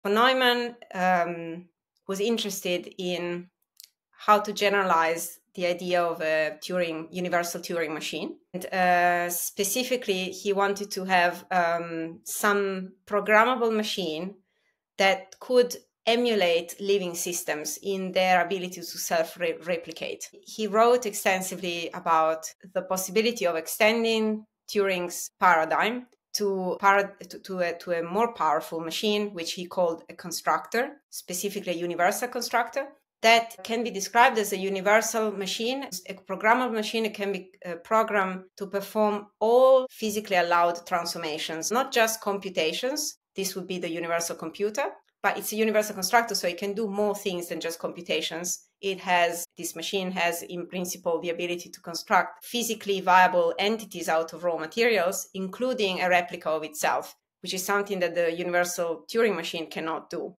von Neumann's Universal Constructor - Beyond Universal Turing Machines
The following is from a conversation about constructor theory, quantum computing & technology, research workflow, and more, with Dr. Chiara Marletto - an Oxford Researcher & Professor & the founding mind of the Constructor Theory.